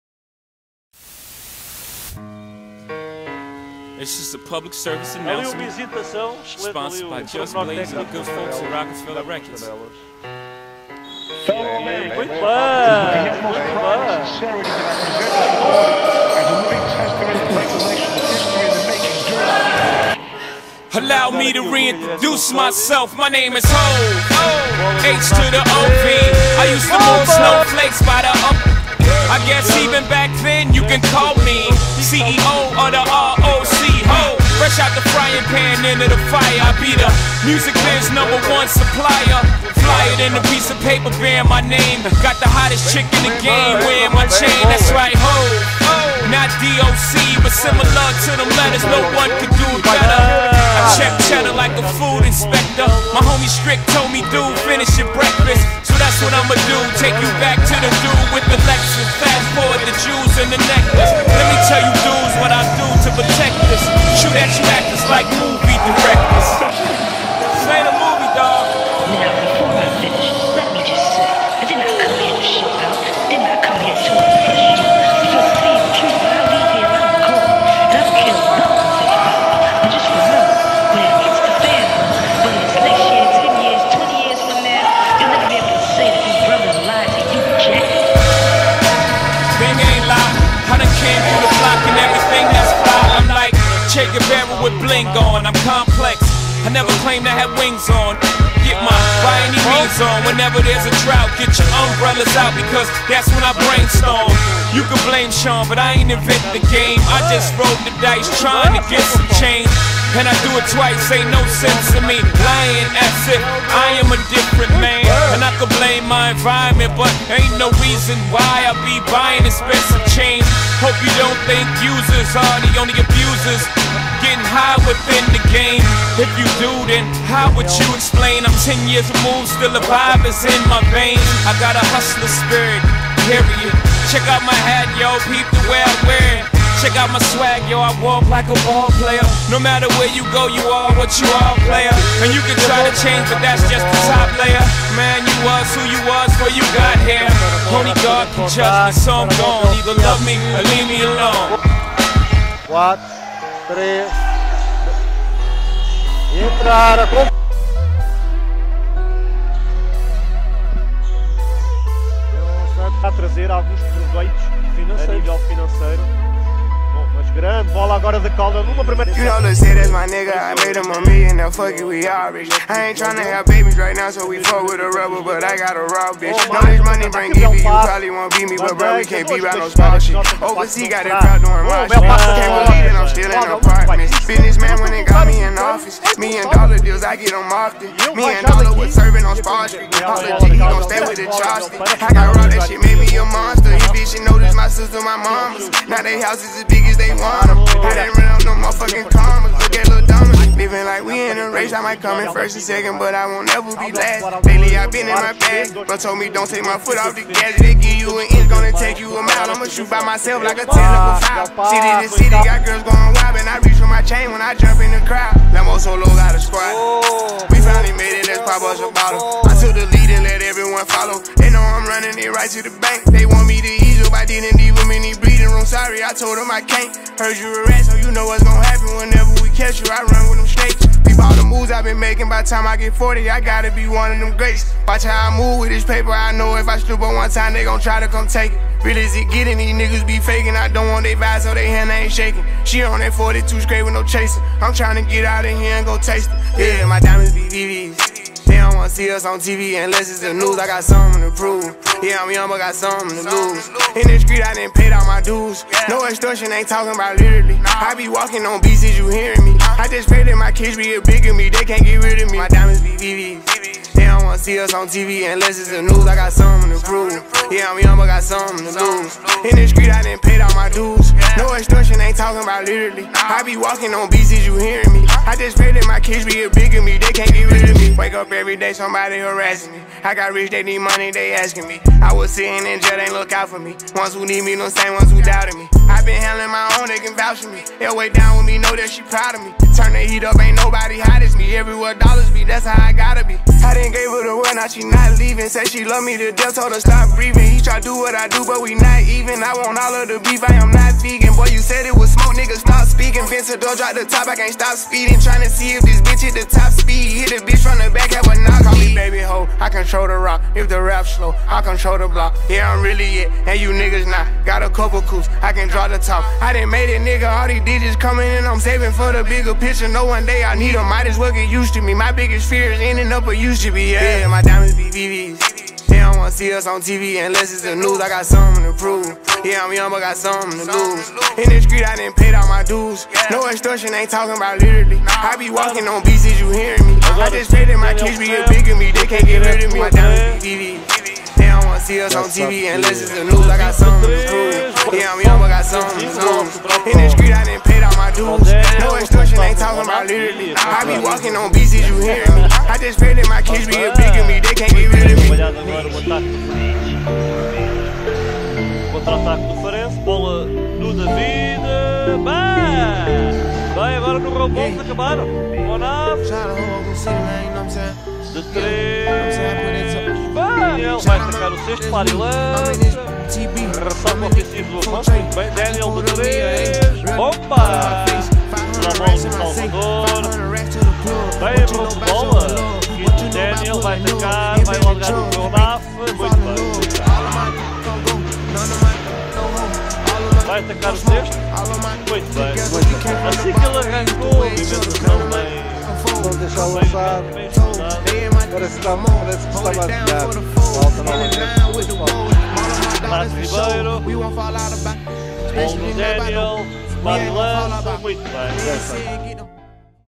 Daniel Caldwell (2023-24 Mid-Season Film)
Daniel Caldwell
Height: 6ft 1in/186cm
Nationality: Canadian
Position: Guard
2023-24 Team: SC Farense (1. Divisao– Portugal)